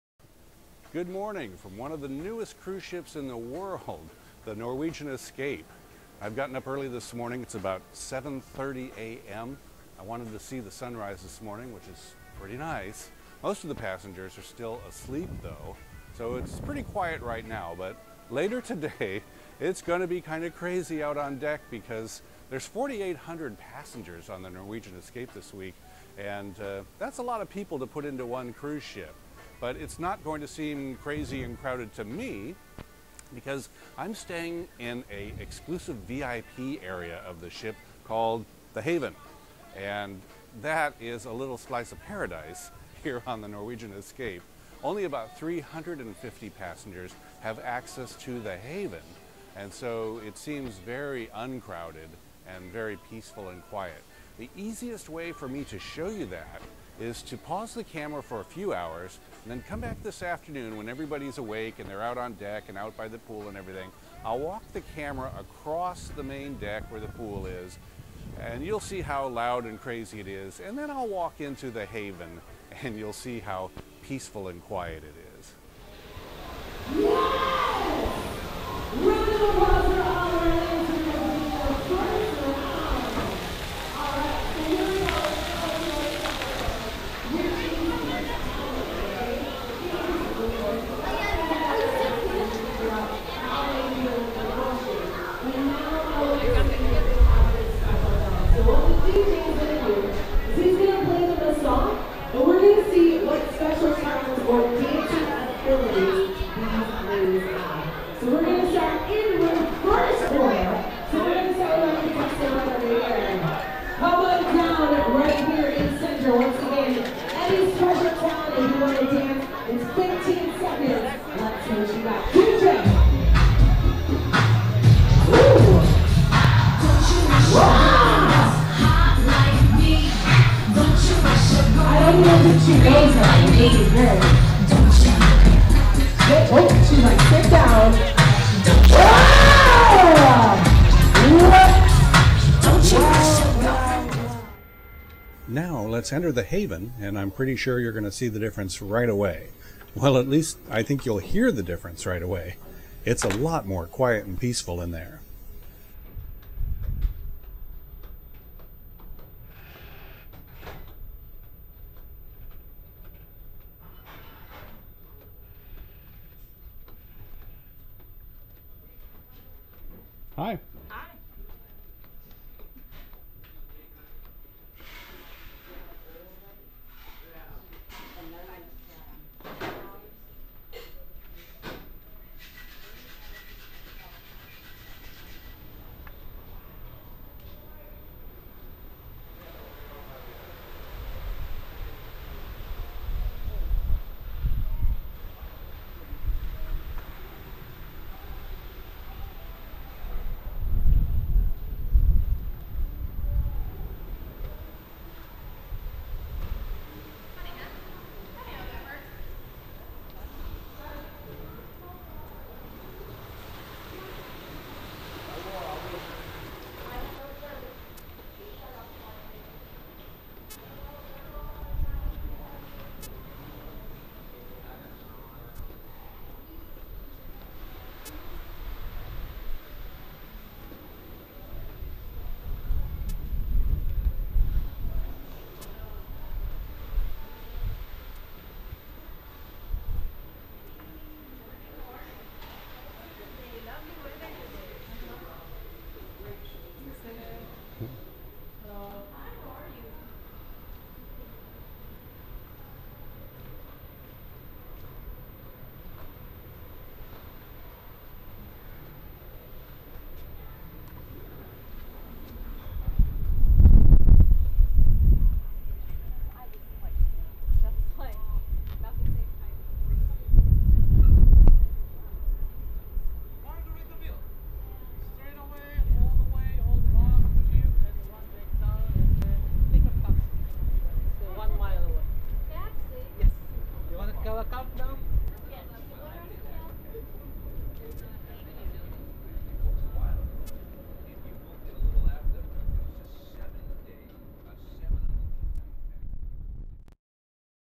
Norwegian Escape - Walking tour of The Haven, Versus "Steerage"!
Cruise ships can be crowded and loud.  This video shows how some people avoid the crowds when cruising on Norwegian Cruise Line...  by paying a premium price to stay within "The Haven", an exclusive VIP area of the ship.

The contrast between the loud event on the pool deck and the peaceful atmosphere within The Haven is amazing!

Also, notice that the people within The Haven are not snobby people in tuxedos drinking martinis.  It's just normal folks who can afford paying a little extra for a peaceful cruise and never having to worry about finding an available lounger or Jacuzzi.

The walking portions of this video were shot with a DJI Osmo motion-stabilized camera.

Shot onboard the Norwegian Escape cruise ship during a sea day, with 4880 passengers onboard.